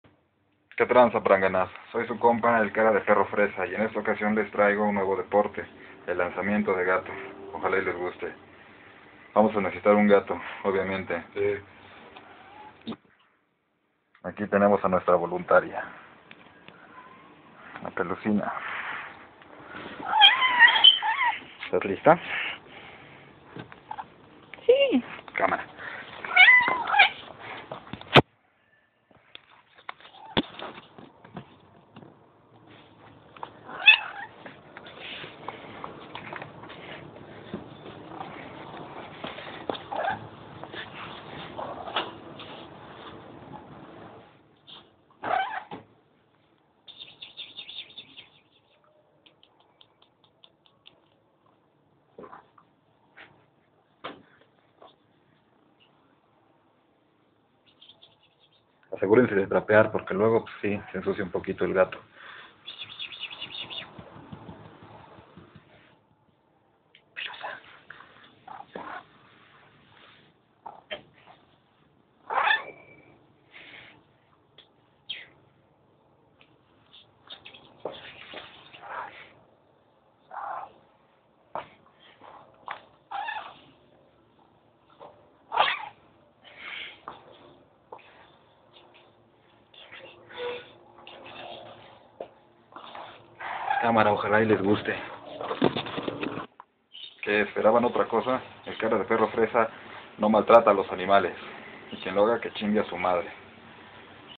el cara de perro fresa lanzamiento de pelusa.
una vez mas el cara de perro fresa nos confirma que es la encarnacion de la inutilidad.